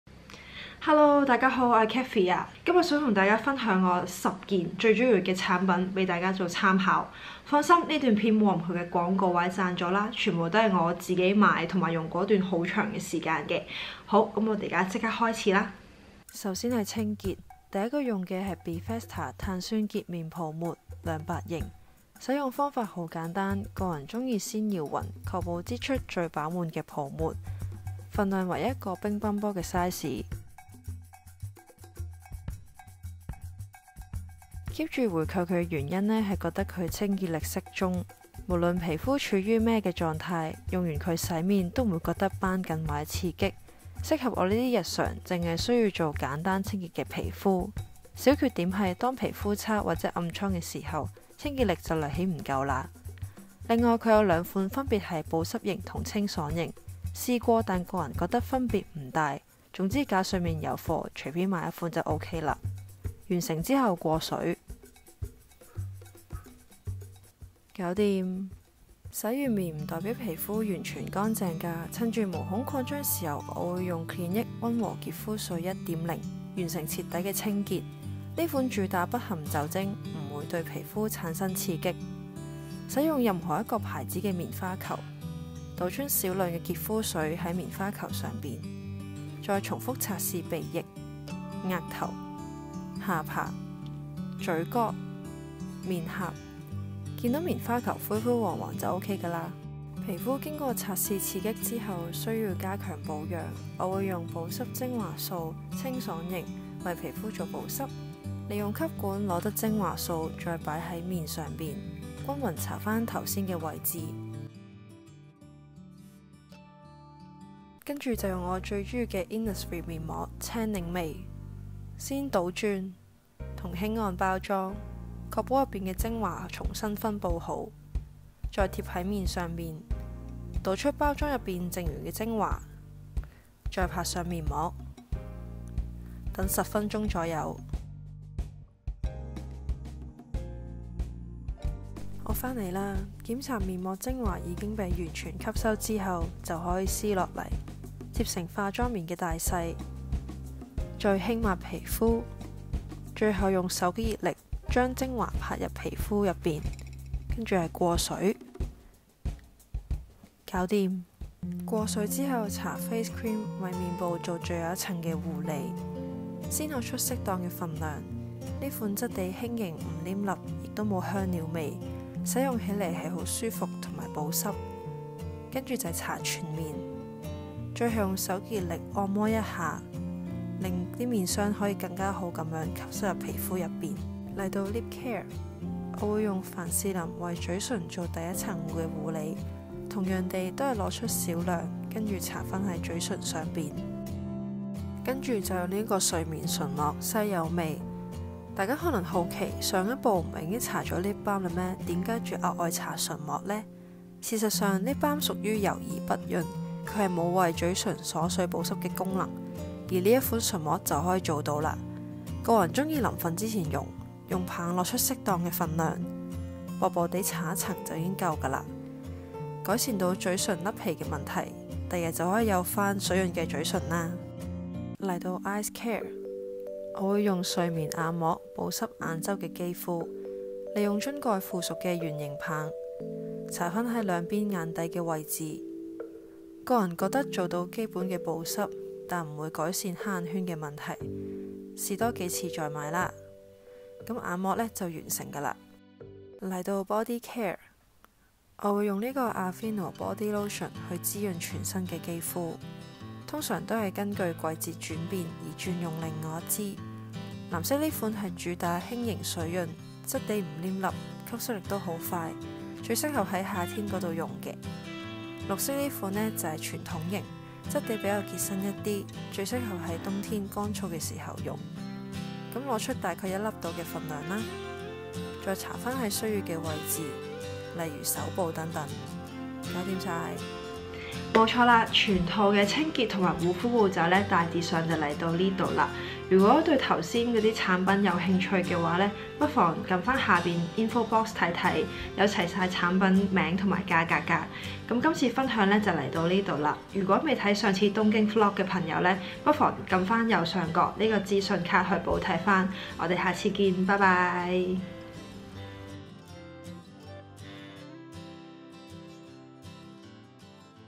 跟我一起護膚吧! 超簡單の5分鐘護膚術 My 5 minutes skincare routine
所有產品都是自己天天在用的，請放心觀賞^_^有需要請自行參閱下面資訊
Last video (中字)Tokyo Journal 東京隨性遊記

Mentioned products:
1. Bifesta 碳酸潔面泡沫 亮白型 ($53/180g)
2. Clinique溫和潔膚水 #1.0 $290/400ml)
3. Ampoula Swiss 保濕精華素
4. innisfree 專屬真萃鮮榨面膜 青檸 ($9/20ml)
5. Lanolin Face Cream ($67/100g)
6. Vaseline - 凡士林純白花士苓 ($20/50ml)
7. Laneige 水潤修護睡眠唇膜 ($130/20g)
8. Laneige 水活循環睡眠眼膜 ($235/25ml)
9. AVEENO® 天然燕麥保濕乳 ($135/350ml)